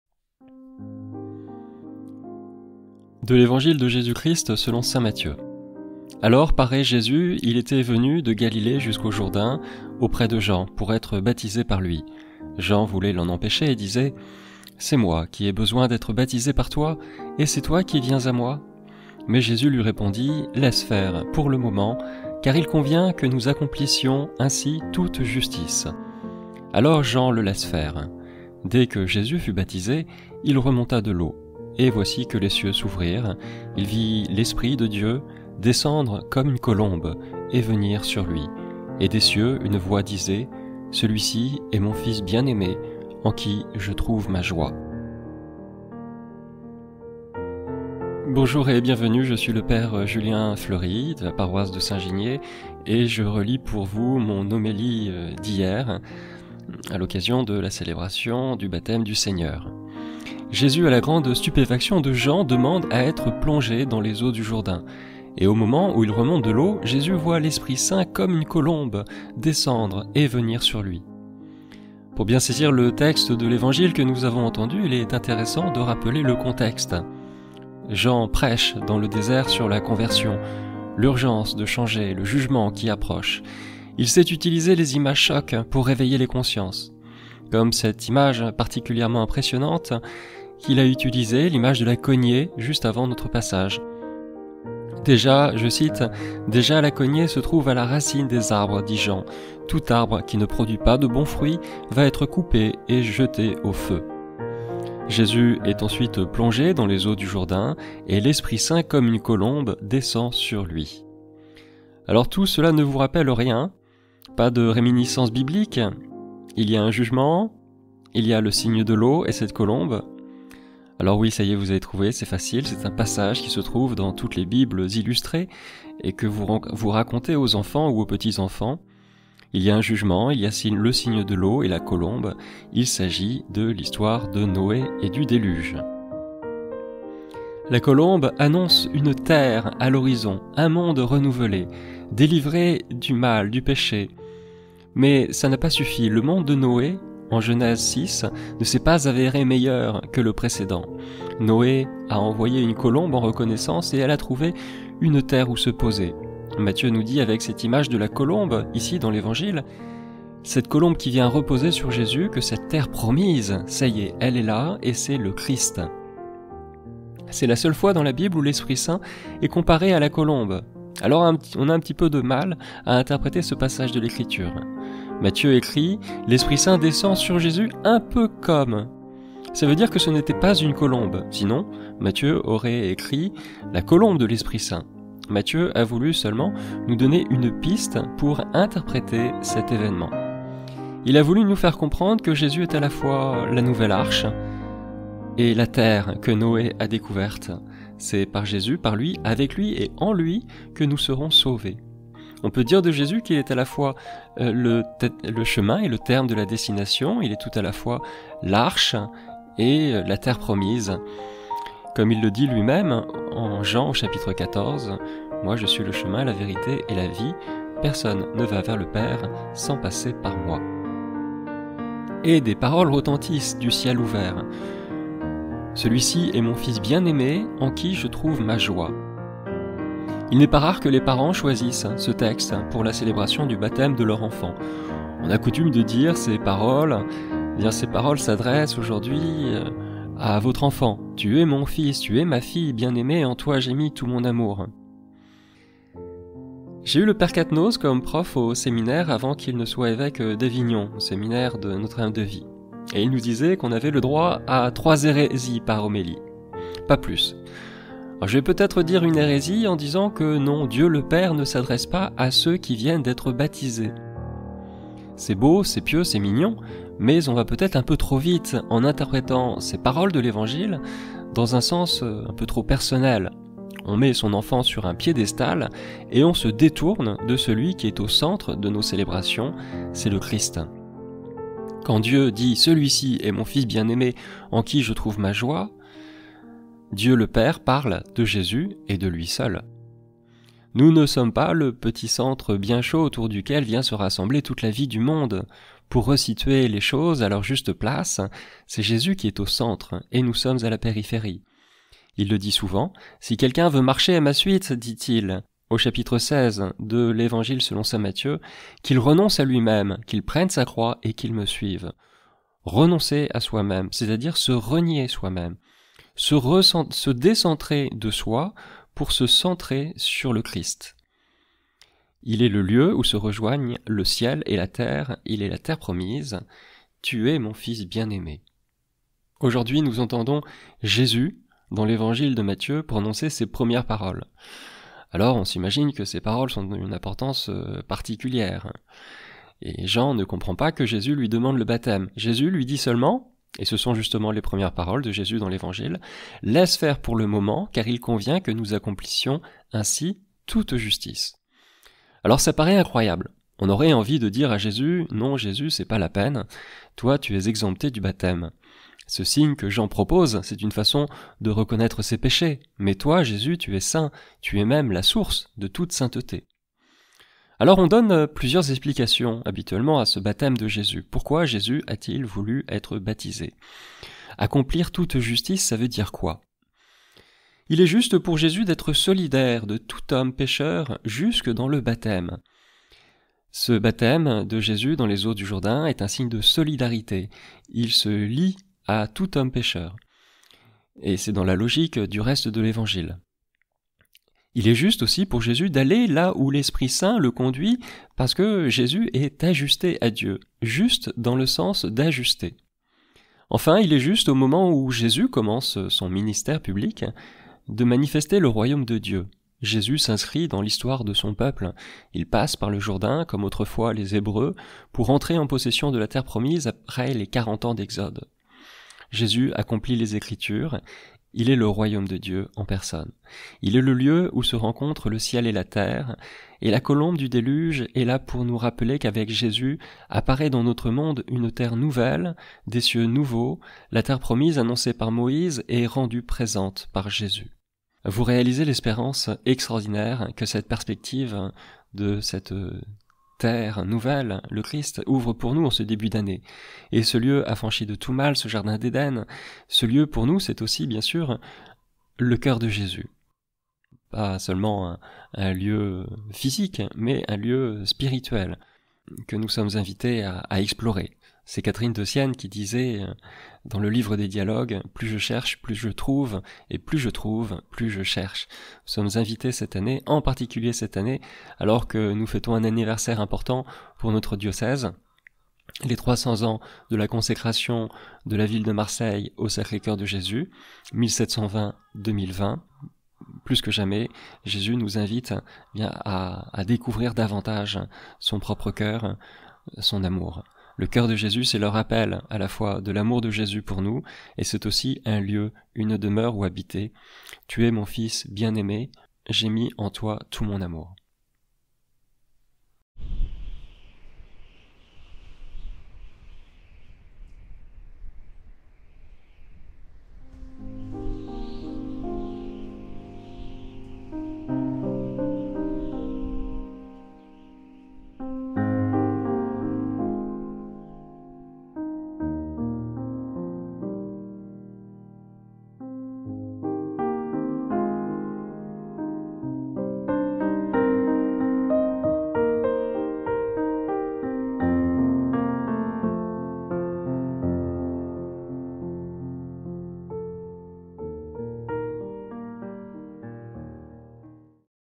Homélie Baptême de Jésus, dimanche 12 janvier 2020, P. Julien (Saint-Giniez, Marseille)
Homélie. Baptême de Jésus, dimanche 12 janvier 2020, P. Julien Fleury (paroisse Saint-Giniez, Marseille). Mt 3,13-17